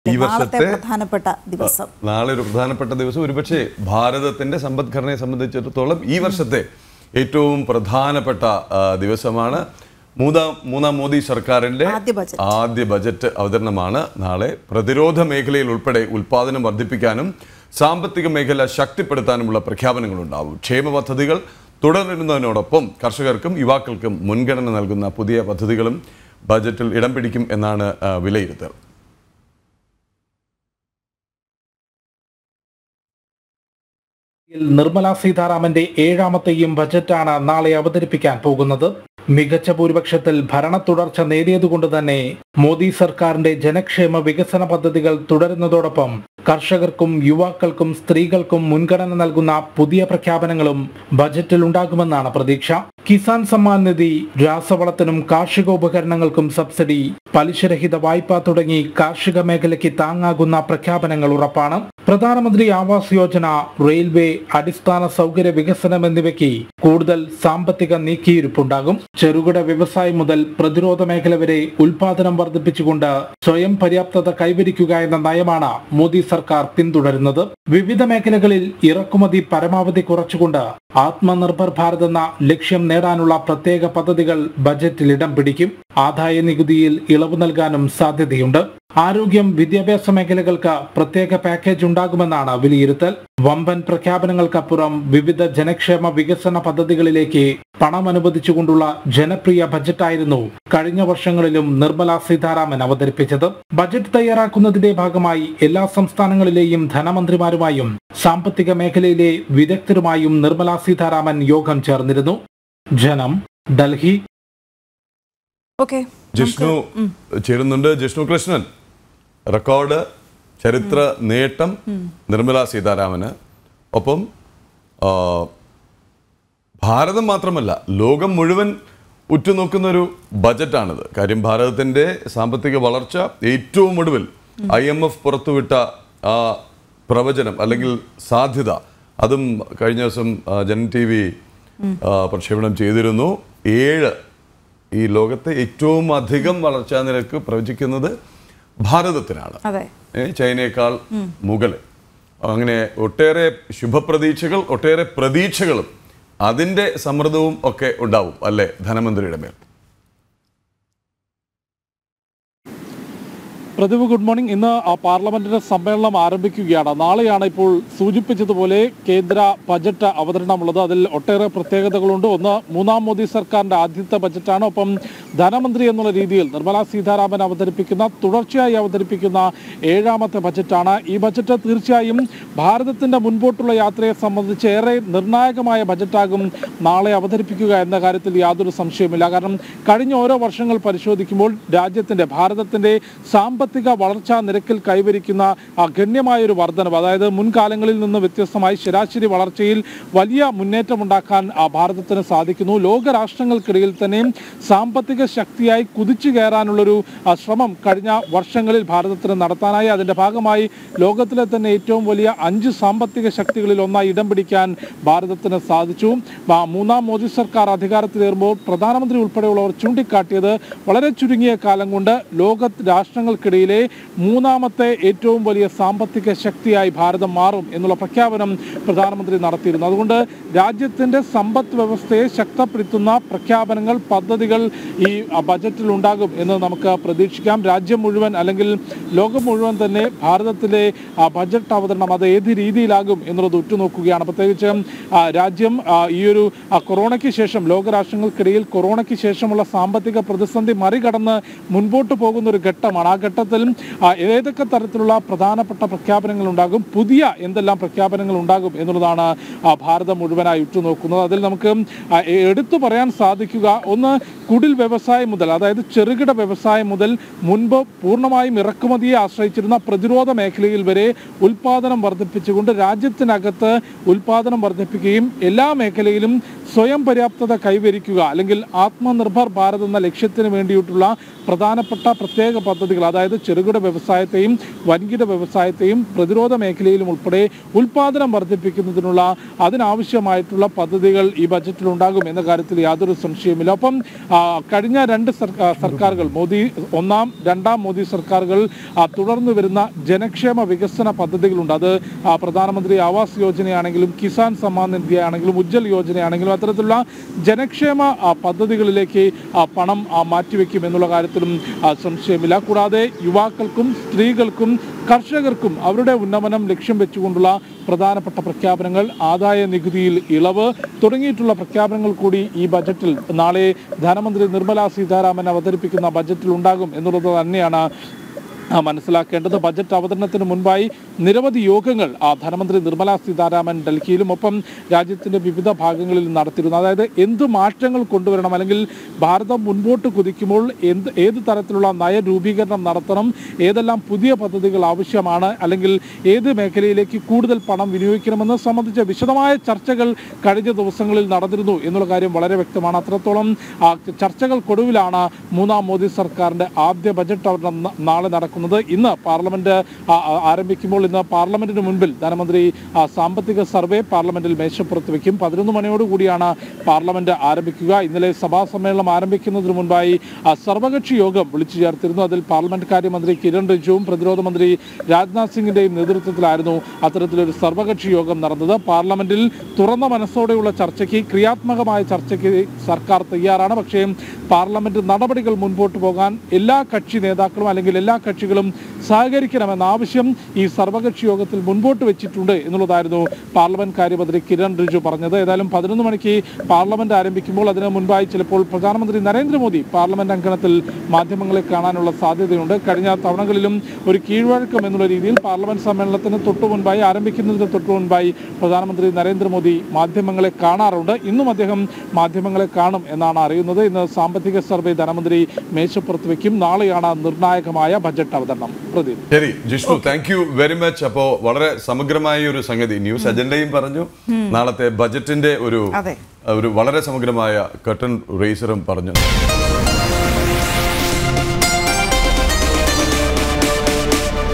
മൂന്നാം മോദി സർക്കാറിന്‍റെ ആദ്യ ബജറ്റ് അവതരണം നാളെ | MODI 3.0 | BUDGET
മൂന്നാം മോദി സർക്കാറിന്‍റെ ആദ്യ ബജറ്റ് അവതരണം നാളെ. പ്രതിരോധ മേഖലയിൽ ഉൾപ്പെടെ ഉൽപാദനം വർദ്ധിപ്പിക്കാനും സാമ്പത്തിക മേഖല ശക്തിപ്പെടുത്താനുമുള്ള പ്രഖ്യാപനങ്ങൾ ഉണ്ടാകും.  ക്ഷേമ പദ്ധതികൾ തുടരുന്നതോടൊപ്പം കർഷകർക്കും യുവാക്കൾക്കും  മുൻഗണന നൽകുന്ന പുതിയ പദ്ധതികളും ബജറ്‍റിൽ ഇടം പിടിക്കുമെന്നാണ് വിലിയിരുത്തൽ.